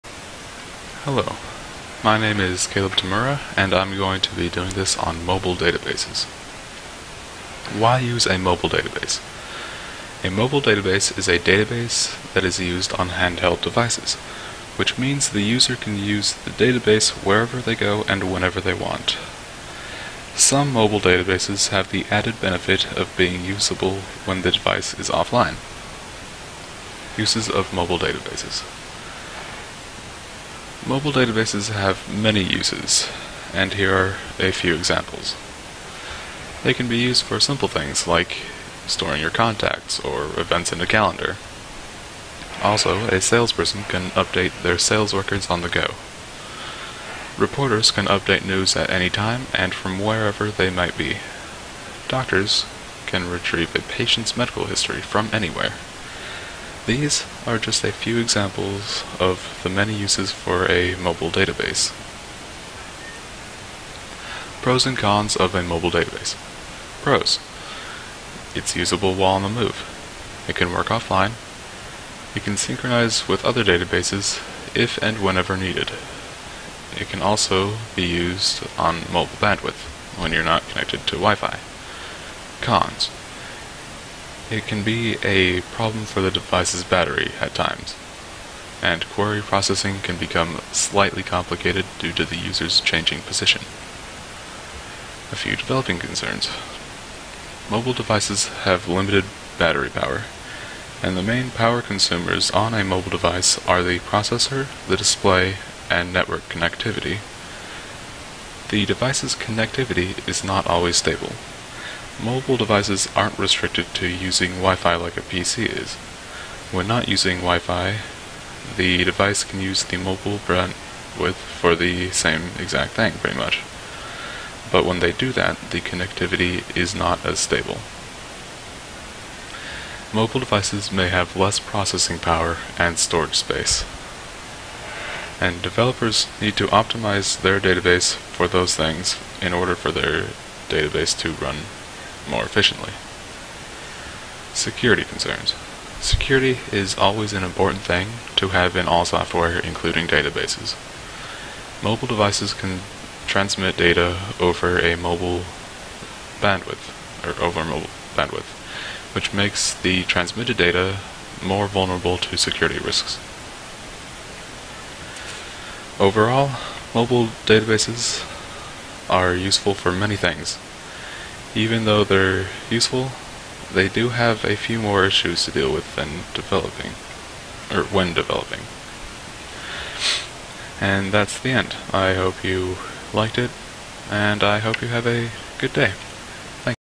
Mobile Databases
A discussion about Mobile Databases, by Caleb Tamura